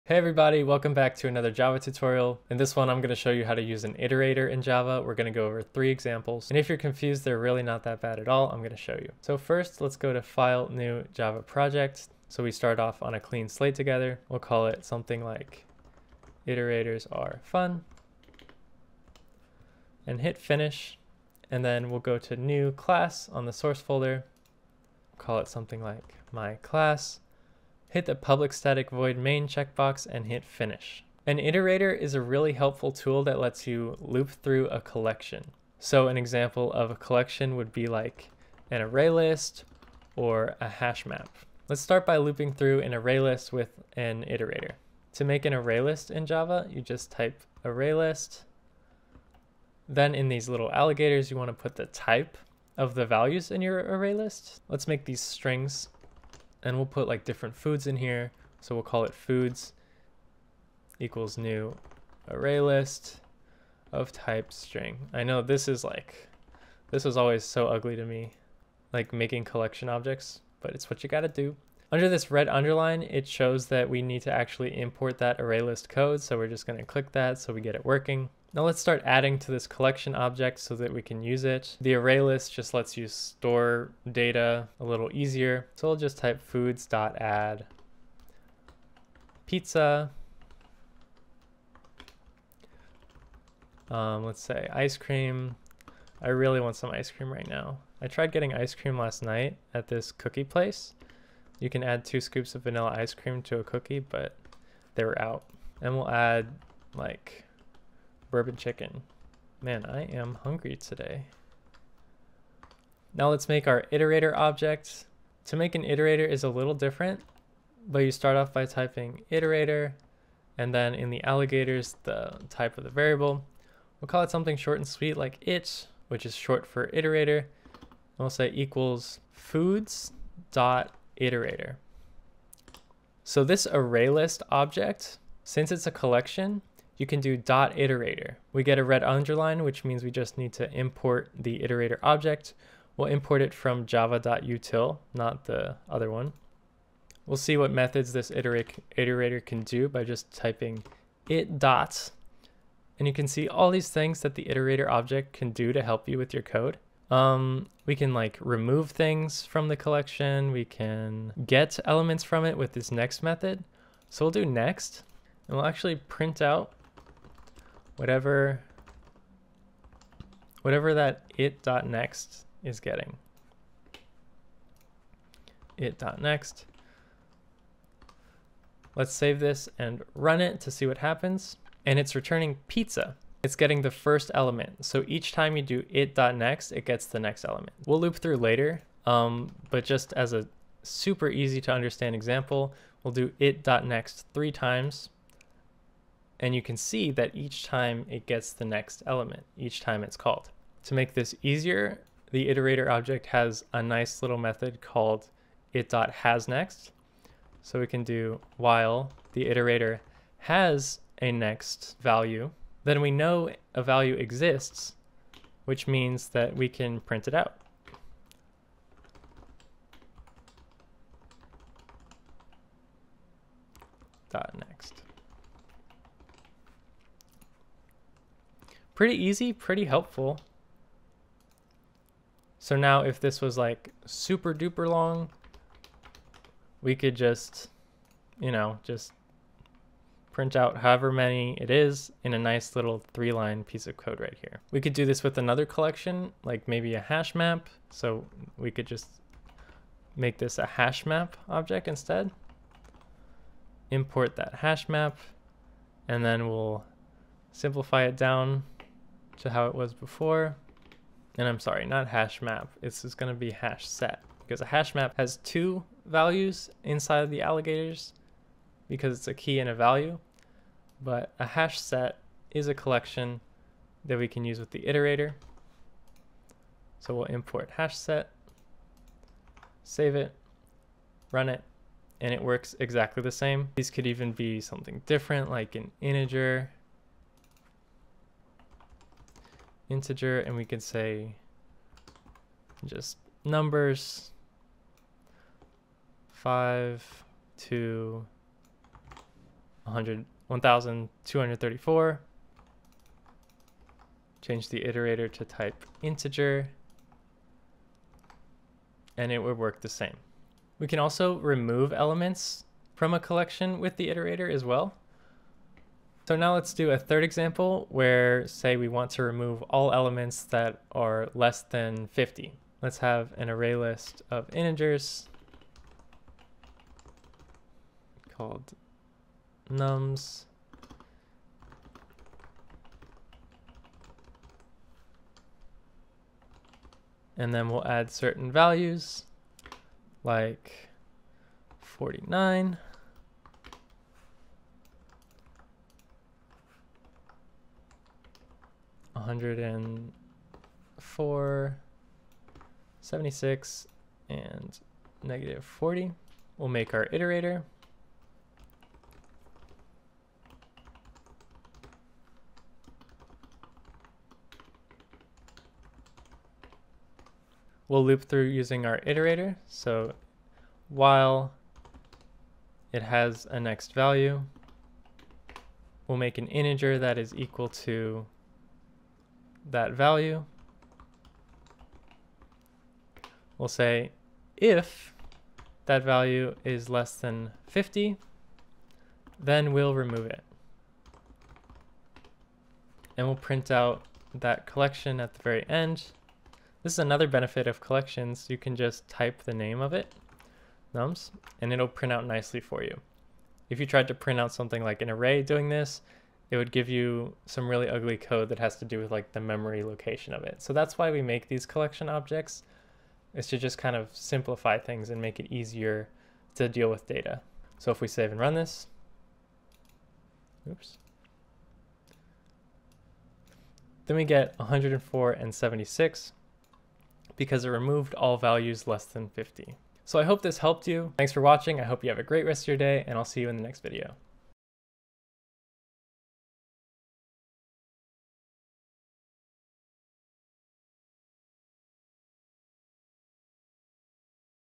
Iterator Java Tutorial #68
$1,500 OFF ANY Springboard Tech Bootcamps with my code ALEXLEE1500. See if you qualify for the JOB GUARANTEE!

In this video, I show you how to use the Java iterator! If you have a collection object like an ArrayList, HashSet or HashMap, you can use .iterator() to create an java iterator object and loop through it and remove values!

I hope you enjoyed this Iterator Java Tutorial!

Alex Lee